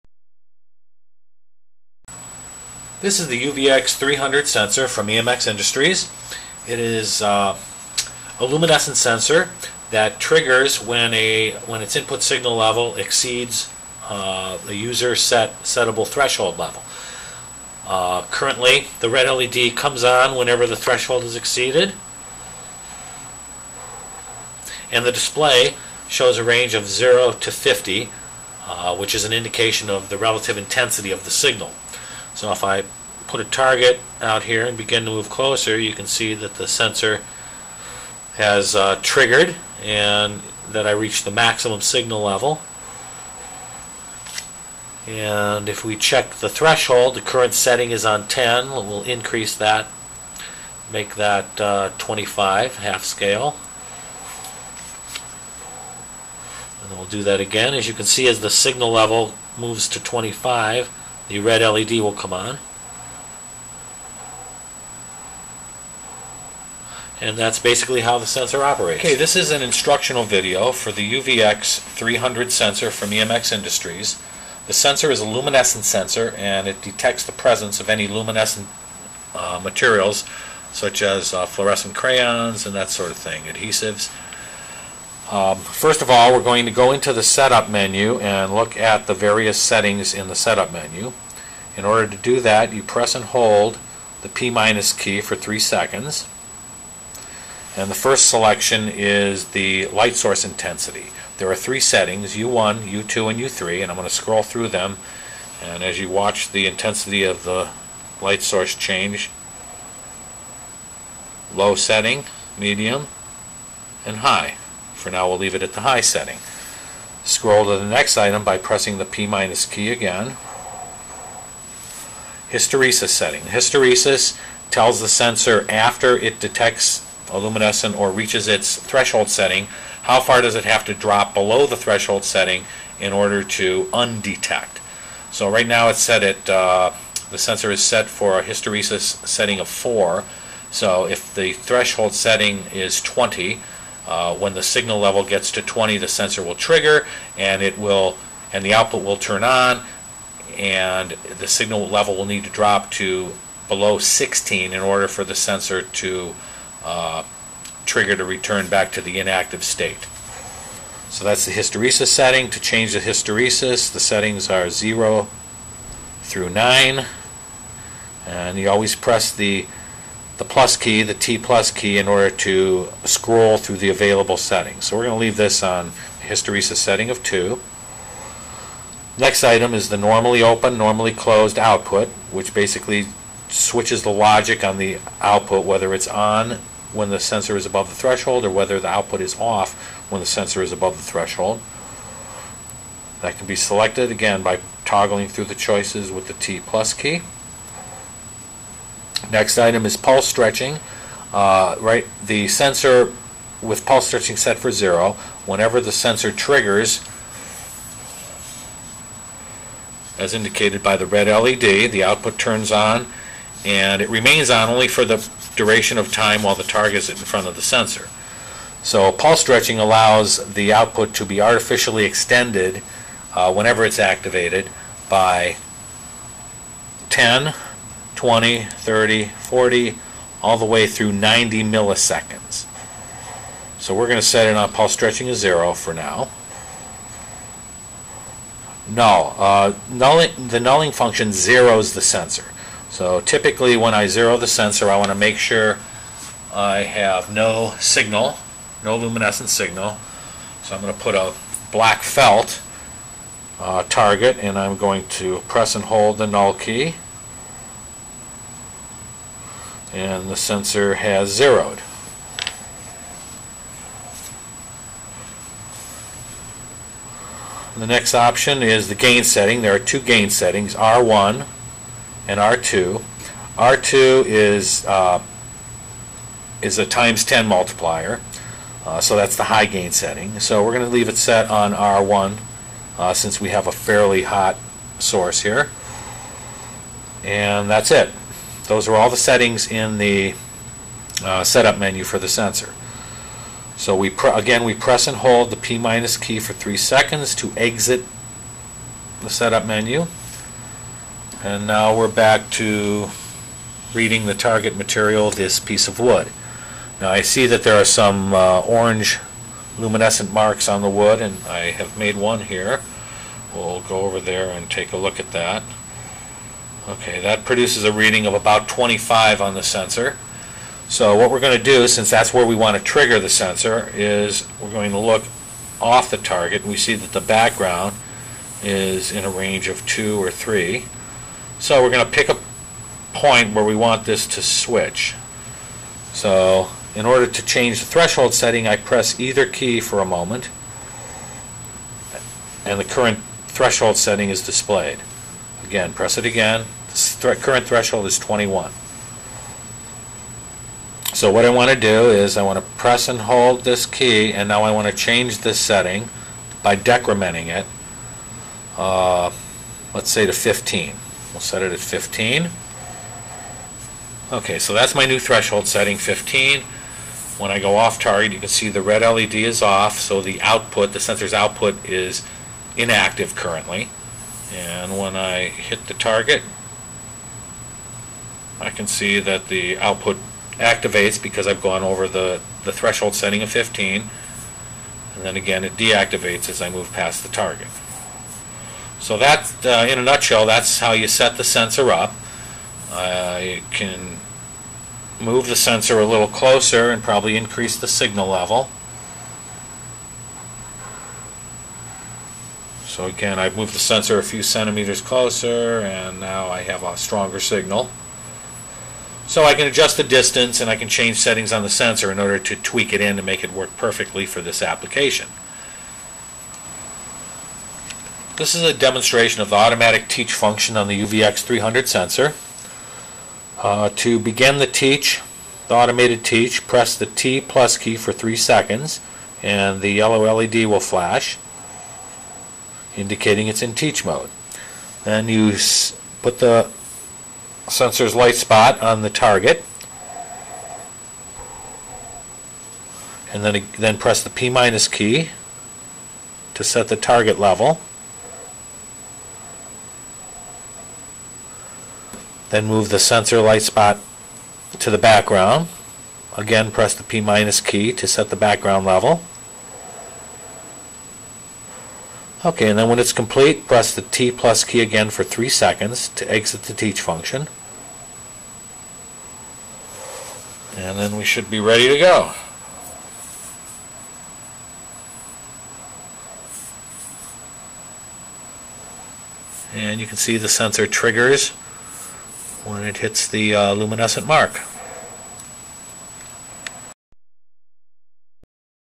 UVX Luminescence Sensor Operating Instructions
UVX Sensor setup and operating instructions.

The UVX series of luminescence sensors are used to verify the presence of UV markings and materials during high-speed production. The UVX series confirms manufacturing processes more reliably than vision systems that cost ten times more.